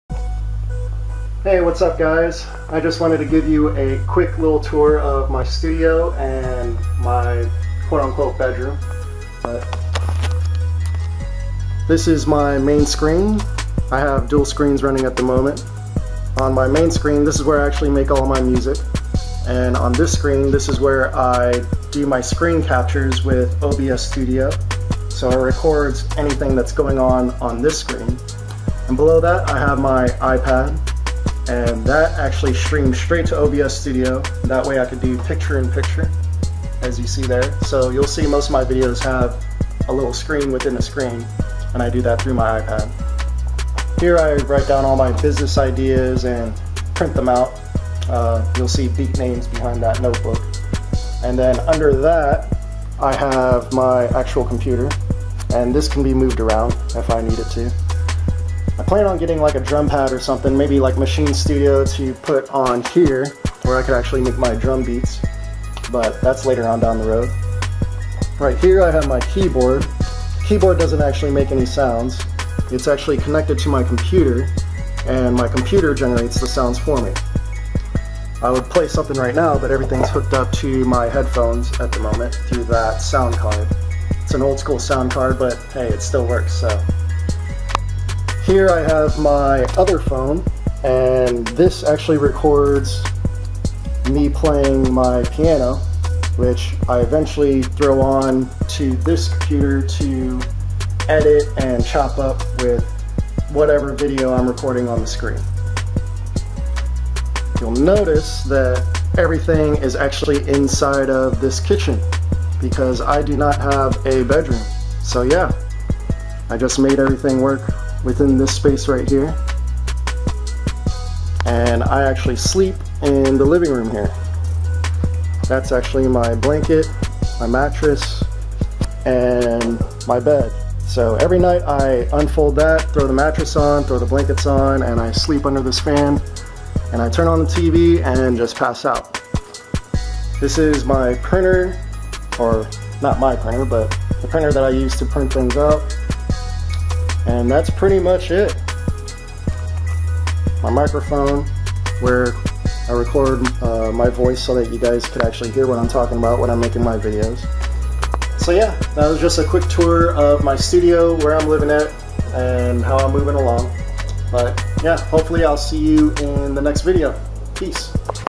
Humble Beginnings: My Home Studio Tour
Just a quick tour of my home studio I made in my kitchen...